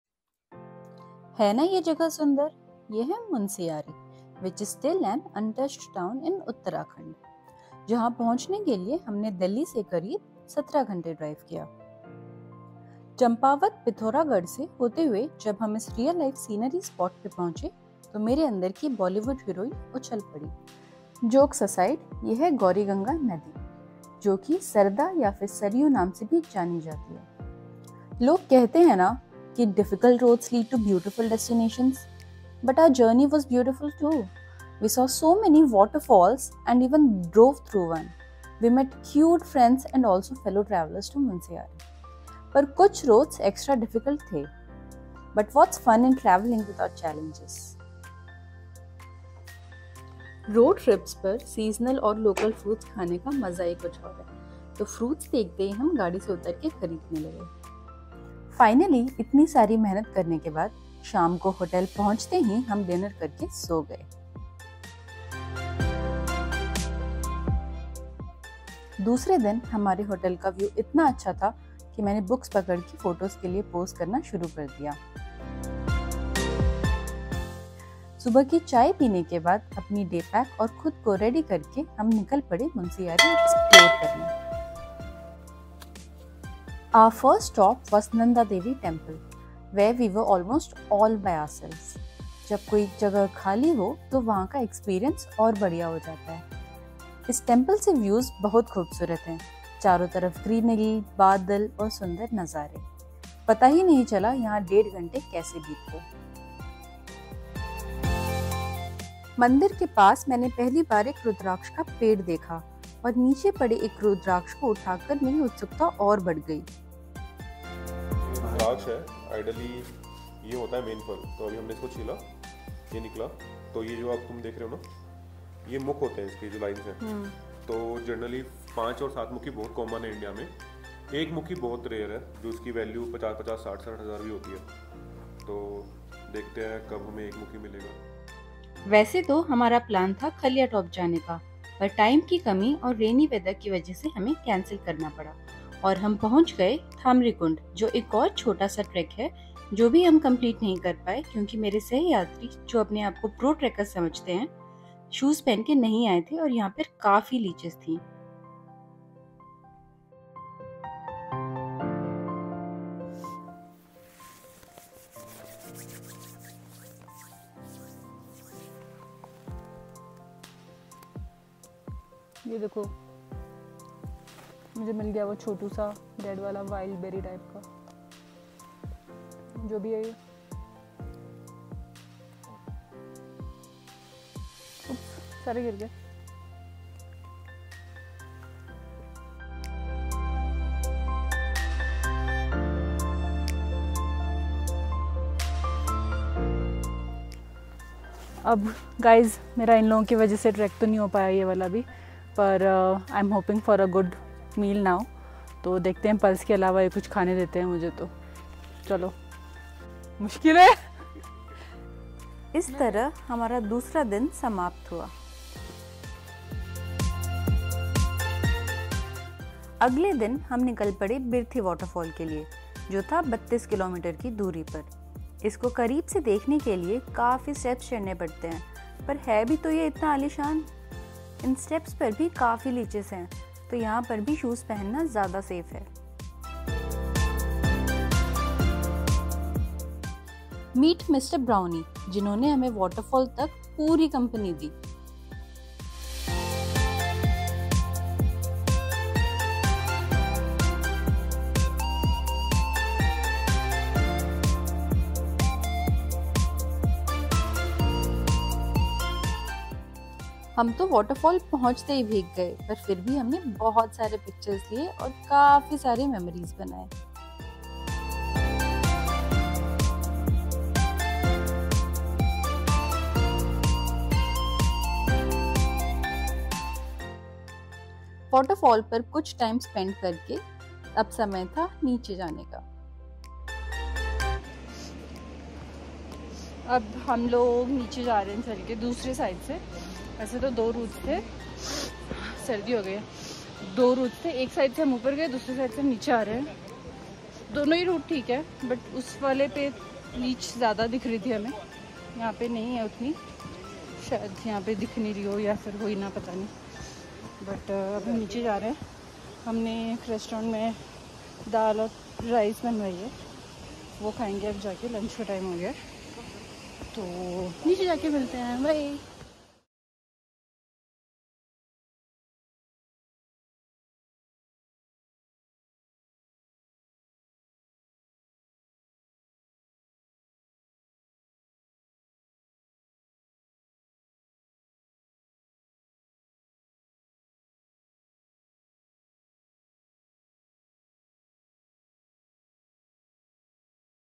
Untouched Beauty of Uttarakhand- Munsiyari- Road Trip Episode 1
We drove 17 hours from delhi to reach Munsiyari in Uttarakhand and the place took my heart. I never imagined a place to be so beautiful.

We visited nanda devi temple, birthi waterfalls and explored a bit more.